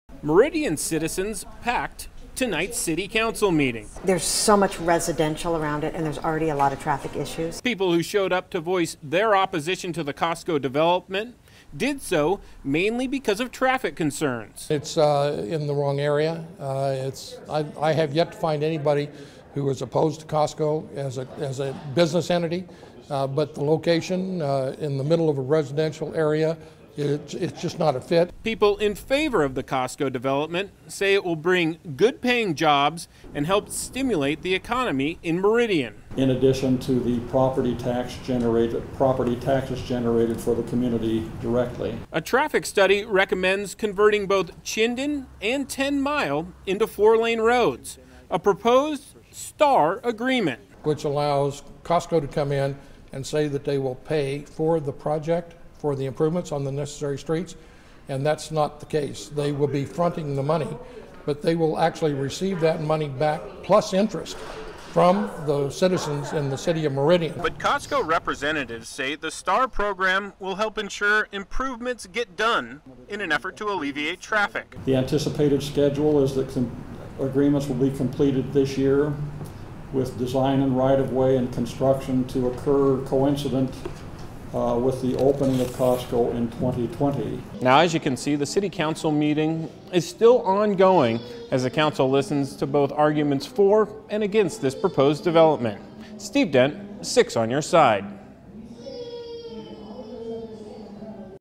Majority of people speak out against Costco development at Meridian City Council meeting
A packed city council meeting